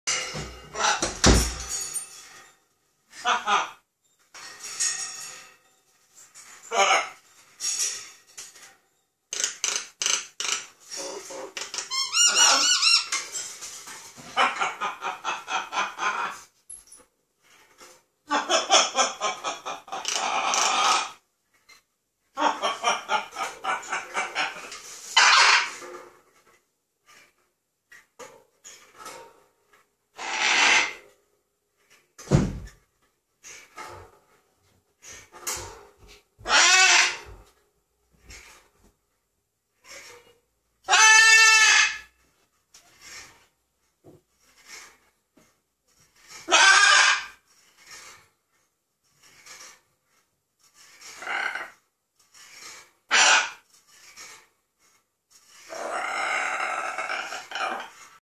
Macaw laughing and making demonic noises
Spicy out 16yro macaw having a good old laugh and then freaking us out by re in acting the exorcist